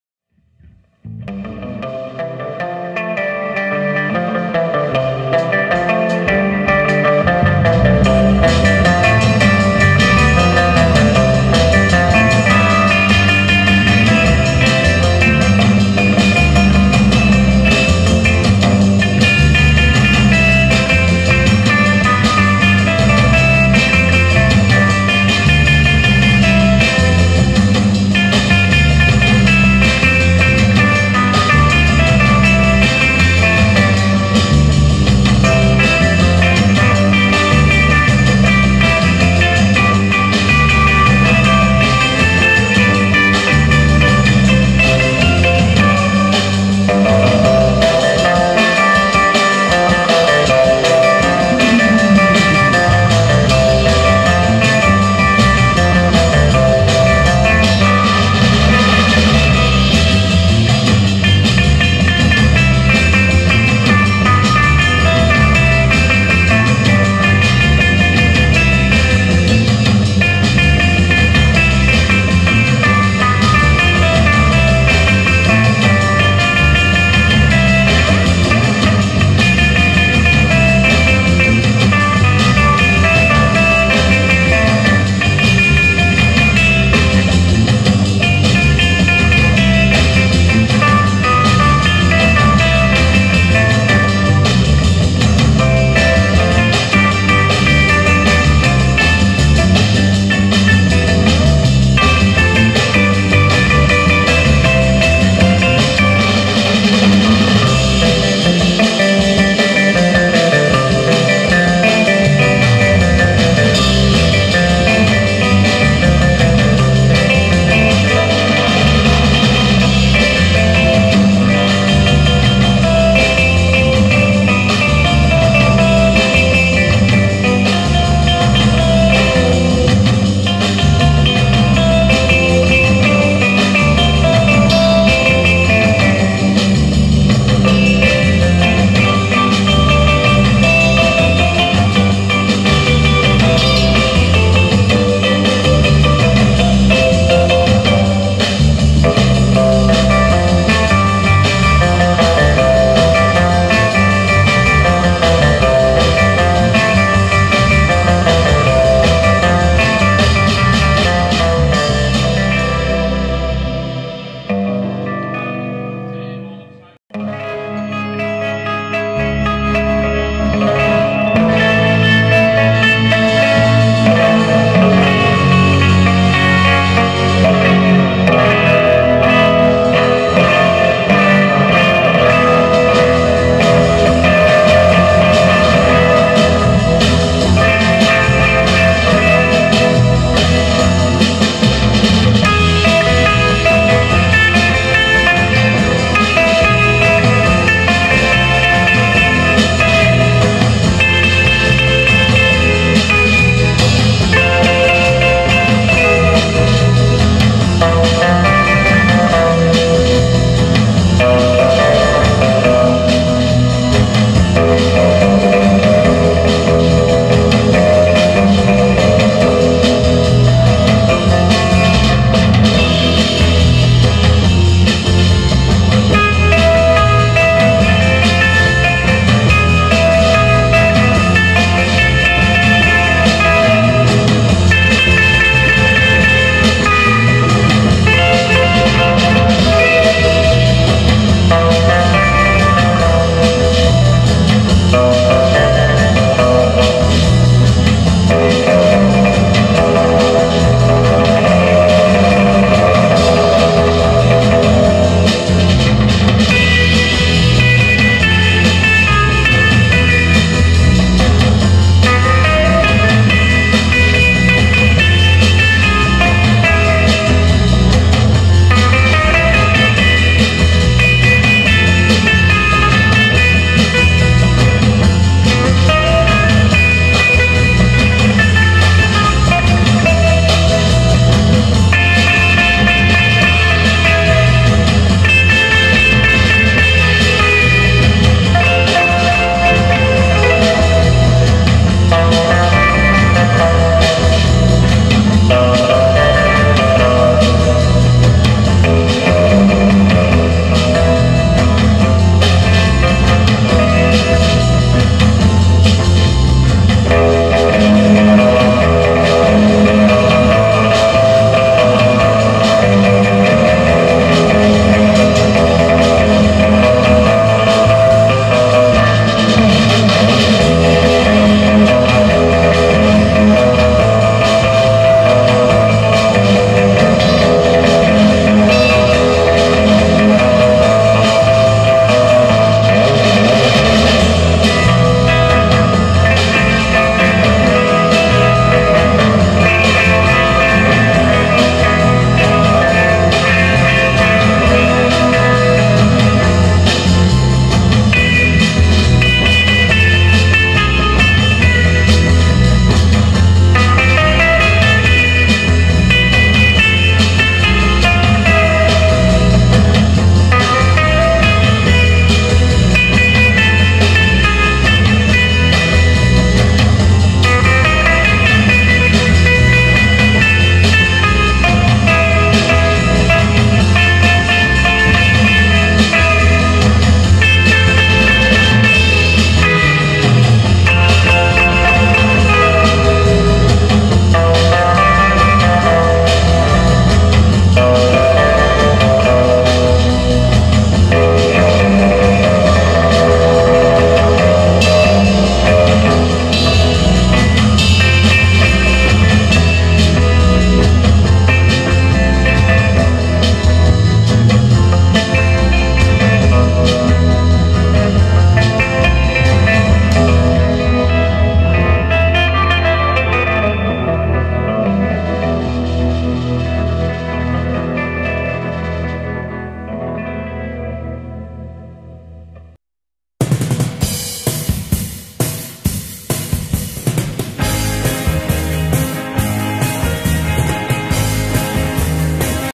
world surfing contest 1968 rincon' PR (the finals)
the world surfing contest 1968 rincon PR with music by the supertones recorded live in rincon 2/22/08 with rob prosky on drums, simon chardiet on bass, and tim sullivan on guitars